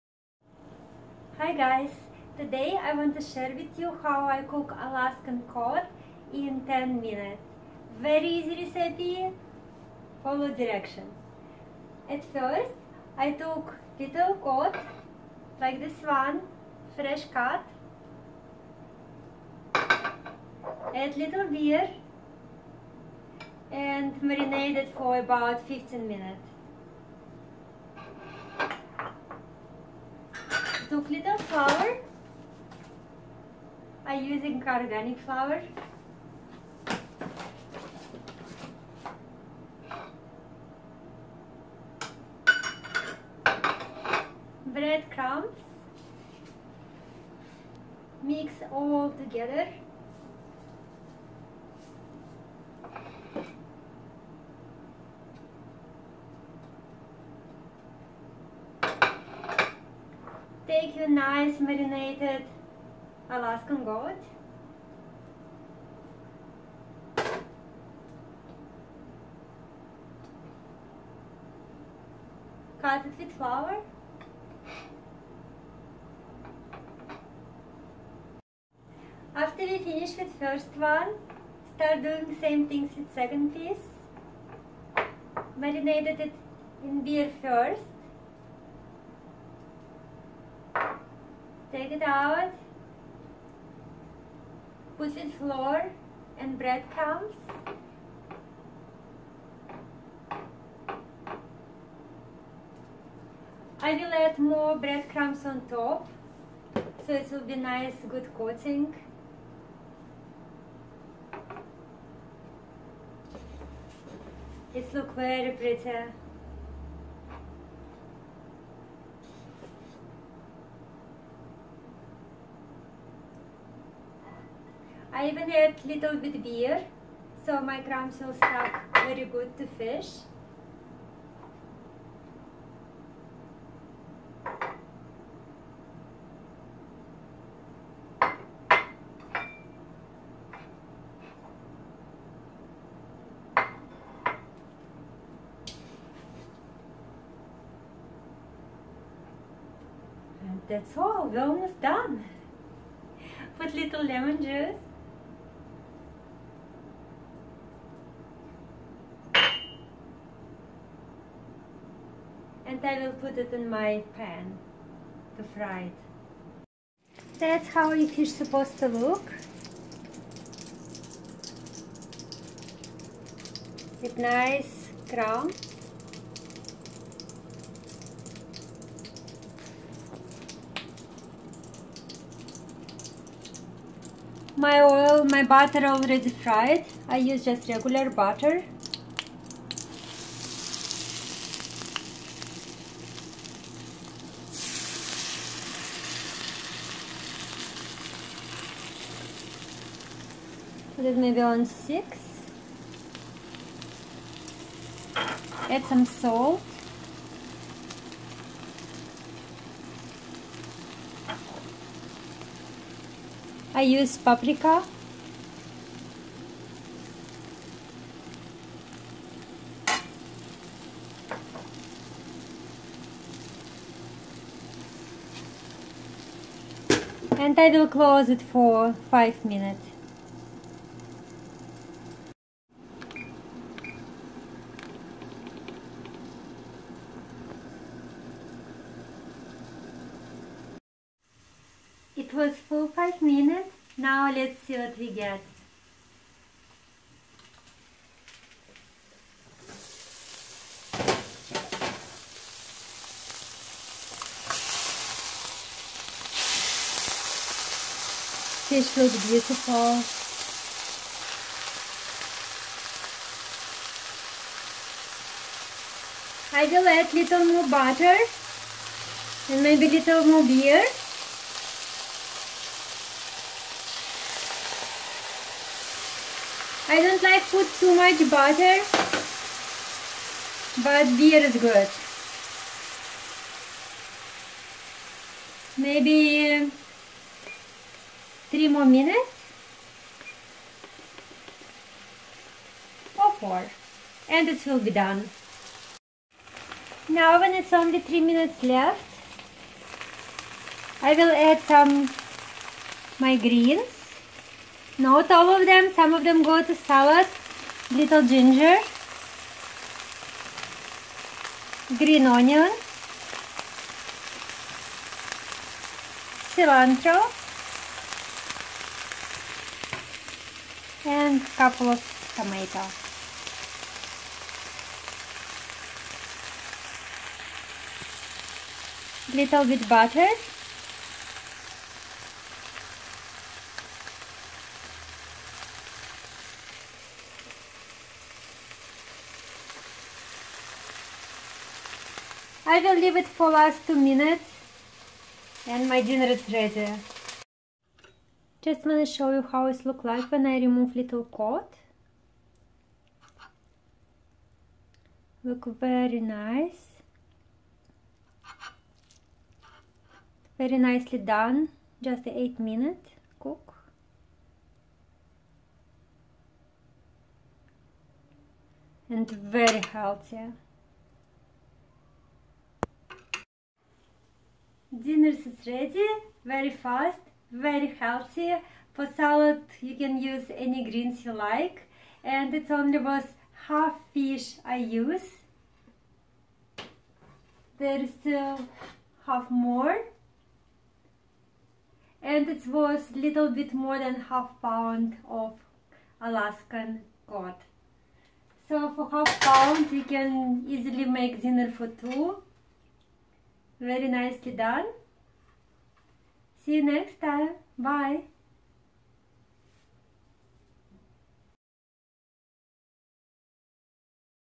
Alaskan Cod. Delicious / Pan Fried
Cooking Alaskan Cod With Beer Battered in 10 minutes. Very easy, healthy ans fast!